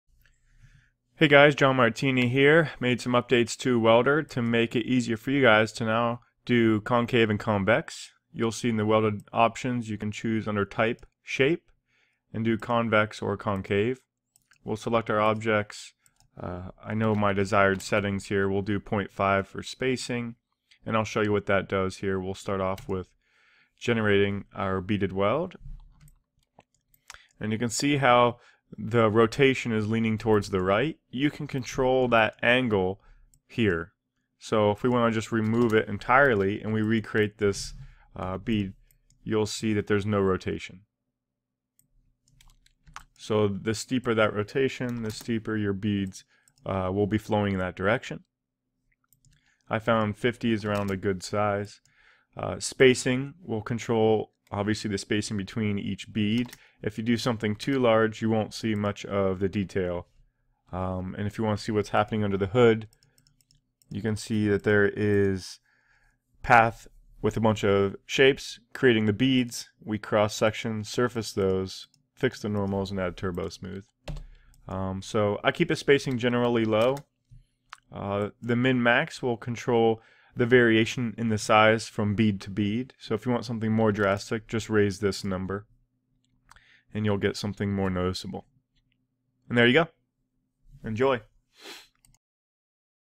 Welder 2 2 1 Concave Convex
In this video I demonstrate the new Concave and convex features in Welder 2.2.1